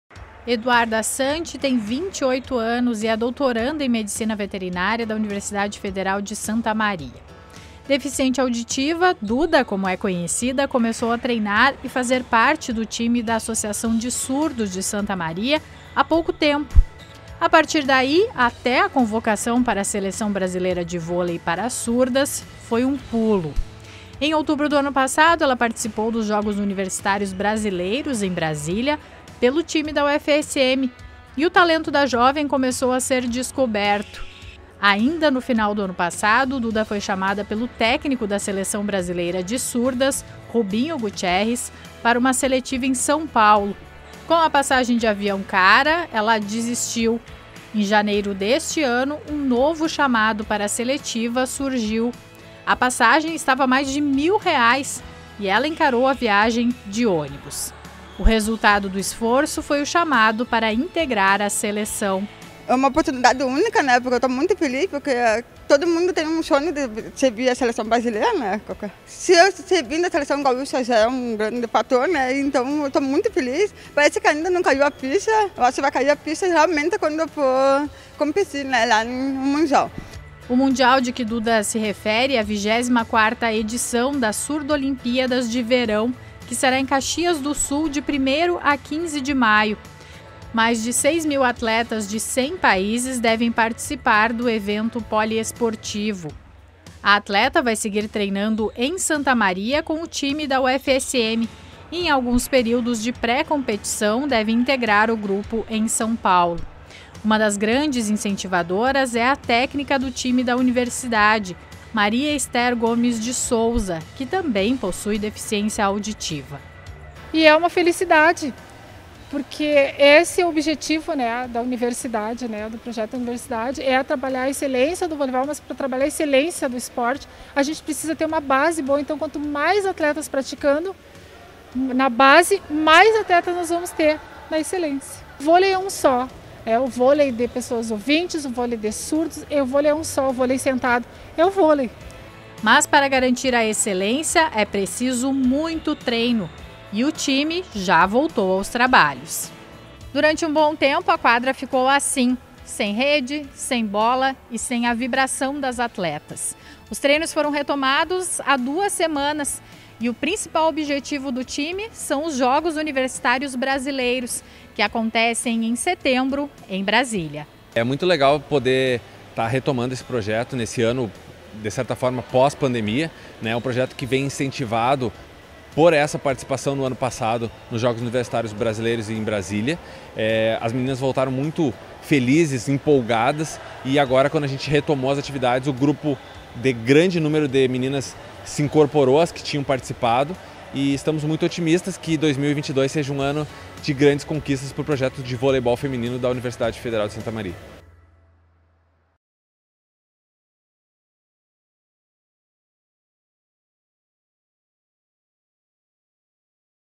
Atleta da UFSM com deficiência auditiva é convocada para a seleção brasileira de vôlei
Eduarda Santi, 28 anos, doutoranda em Medicina Veterinária da Universidade Federal de Santa Maria (UFSM), jogava futebol americano há cinco anos quando foi chamada para começar a jogar vôlei. Deficiente auditiva, Duda, como é conhecida, começou a treinar e fazer parte do time da Associação de Surdos de Santa Maria (ASSM). A partir daí, até a convocação para a seleção brasileira de vôlei para surdas, foi um pulo.